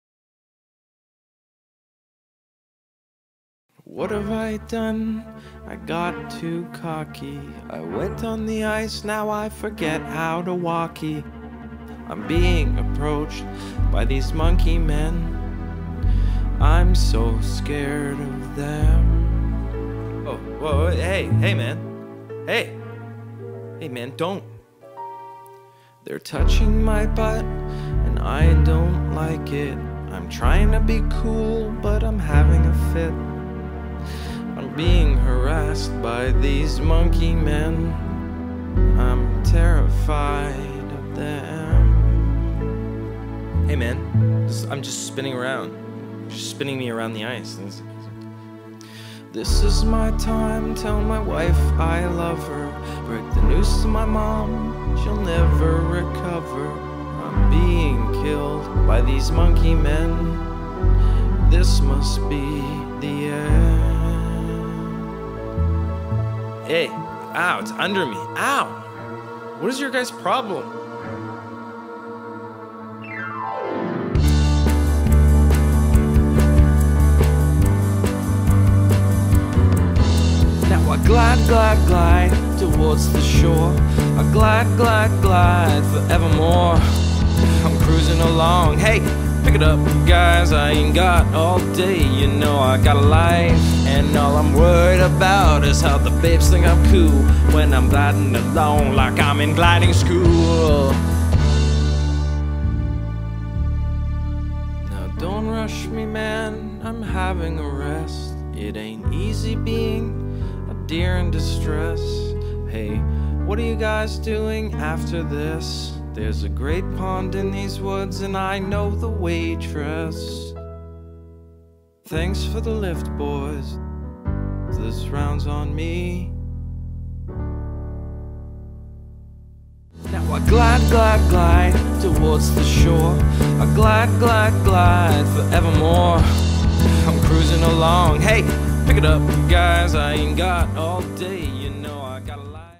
Deer On Ice Rescued: The Musical
A deer trapped on slippery ice sings a little song